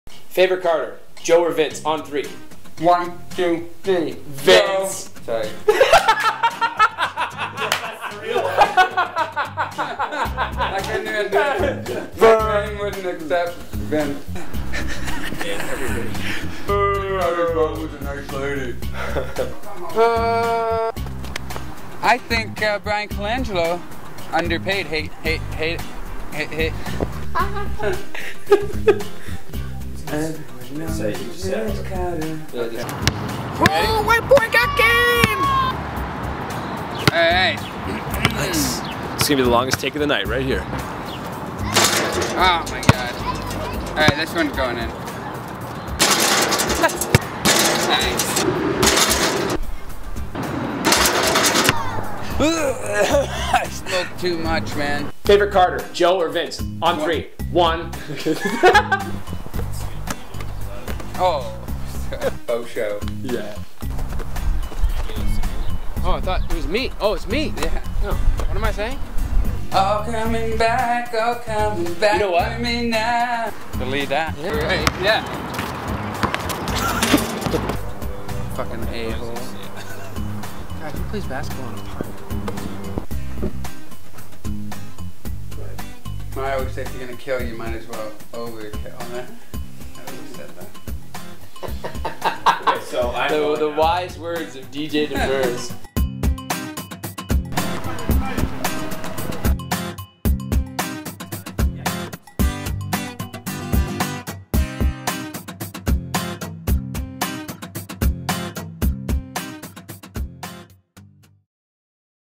Sh*t Raptors Fans Never Say - Blooper Reel
Bloopers from the shoot for 'Shit Raptors Fans Never Say'.